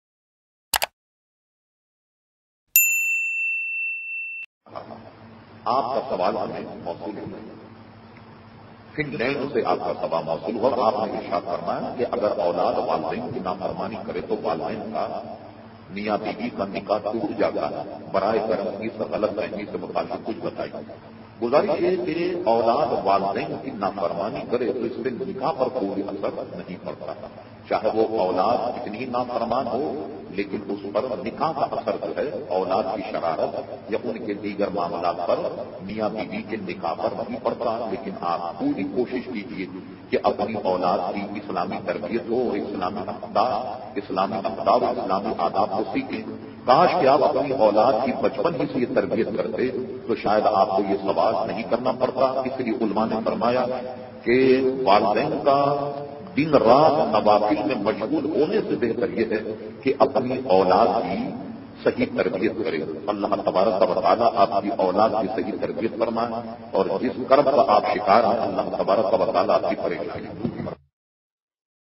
Aulad ki Tarbiyat || اولاد کی تربییت ||Syed Shah Turab ul Haq Qadri
Mard e Momin Mard e Haq Peer e Tareeqat Hazrat Allama Syed Shah Turab ul Haq Qadri's Speeches / Bayan / Lectures / Question and Answers Session.